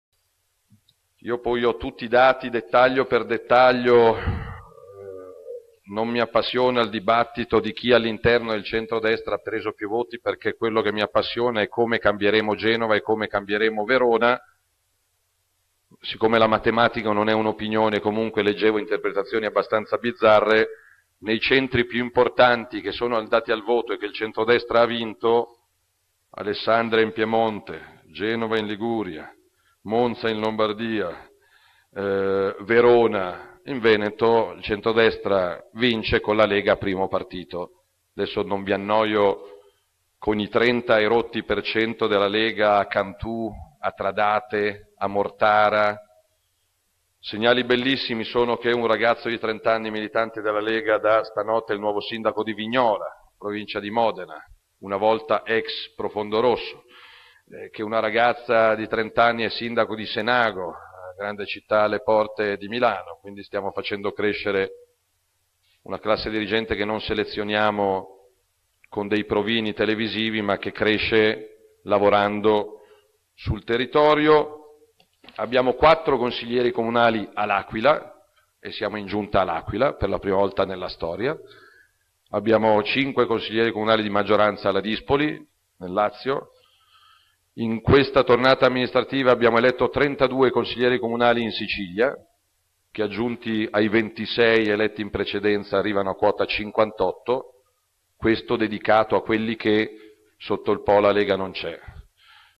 Amministrative, Salvini: Lega partito di centrodestra con piu' voti nei grandi centri
(Agenzia Vista) Milano, 26 giugno 2017
Matteo Salvini, leader della Lega Nord, ha tenuto una conferenza stampa alla sede del patito per commentare l'esito delle elezioni amministrative. Una vittoria del centrodestra e in particolare della Lega, ''che nei grandi centri e' stato il partito di centrodestra con il maggior numero di voti'', ha affermato Salvini. fonte Facebook